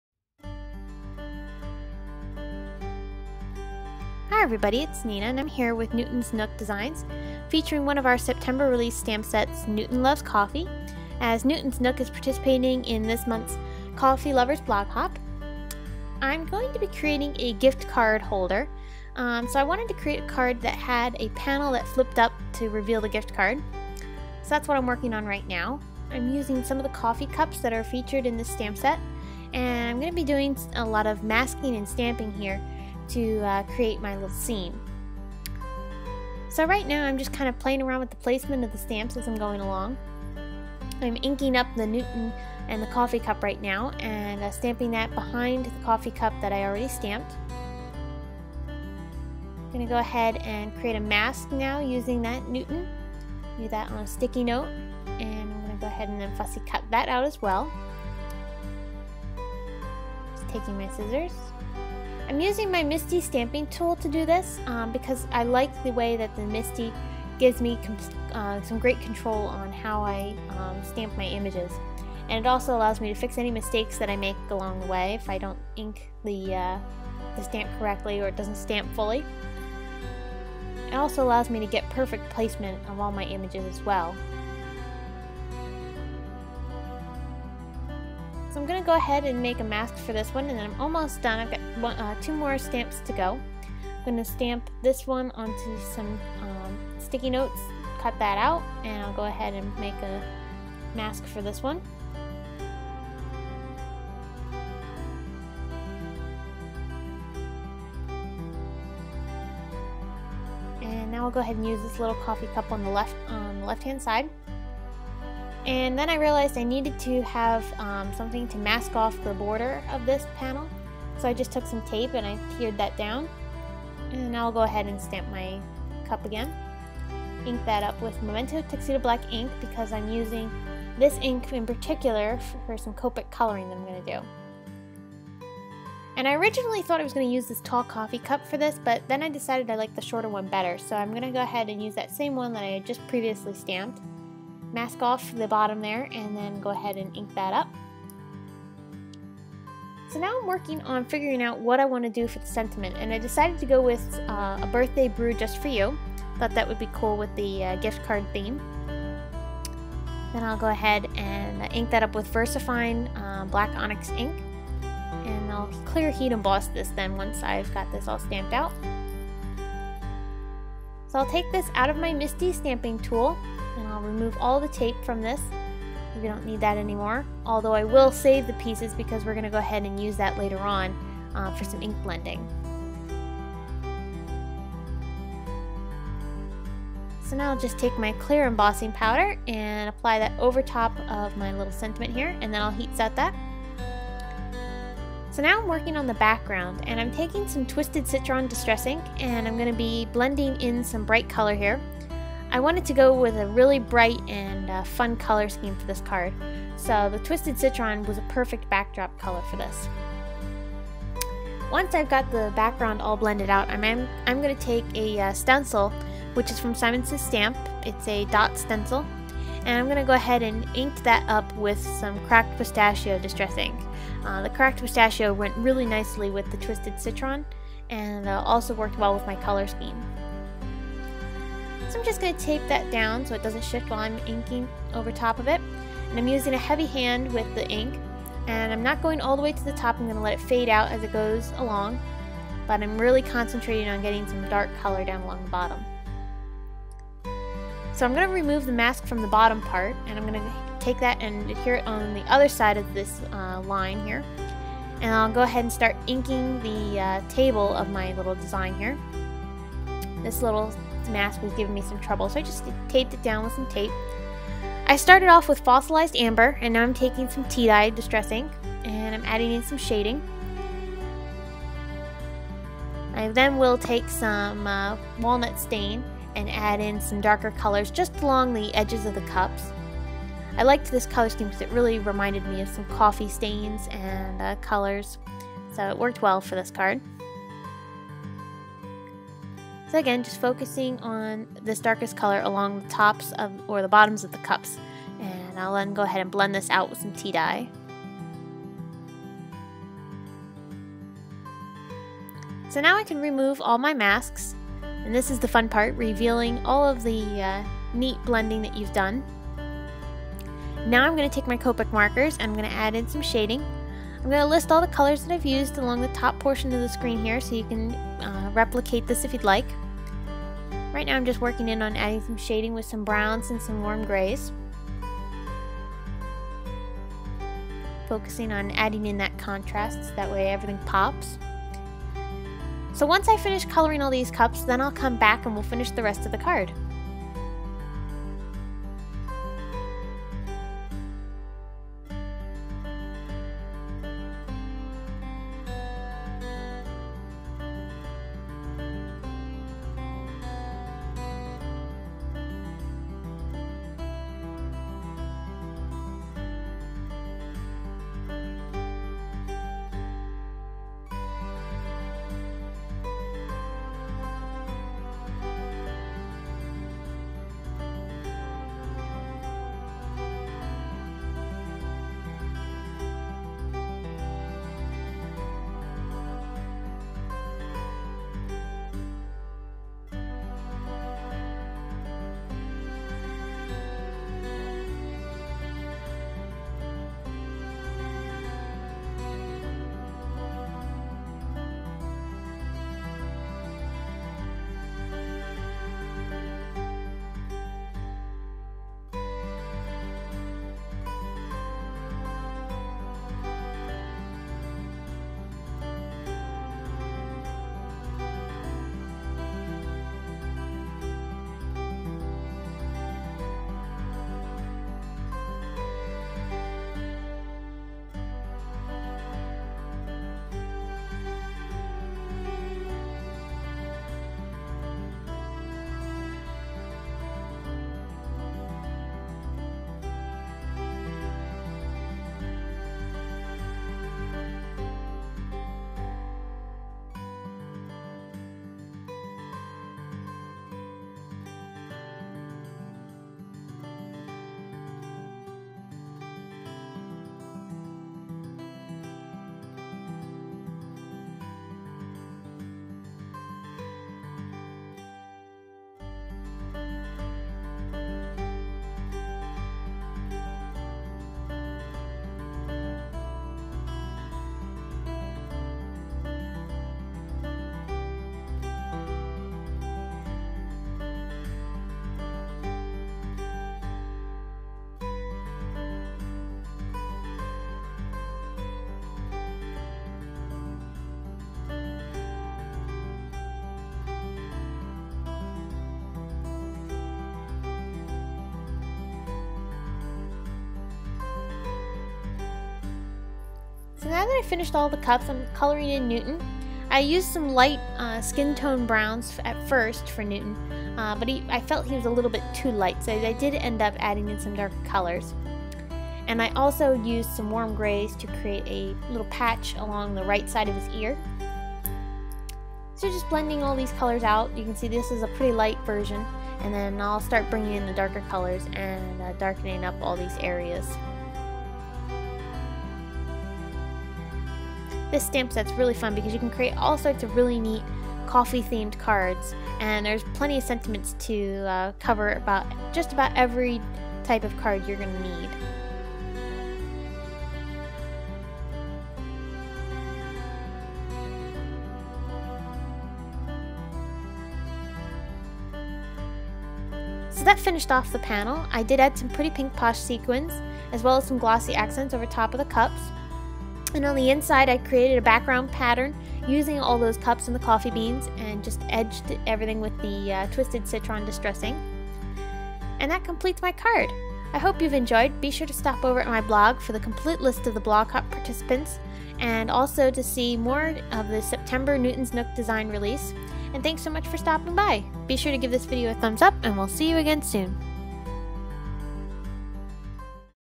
Coffee Lovers Blog Hop Copic Colored "Gift" Card
Gift Card Holder using the Newton's Nook Designs "Newton Loves Coffee" stamp set, and in participation of the September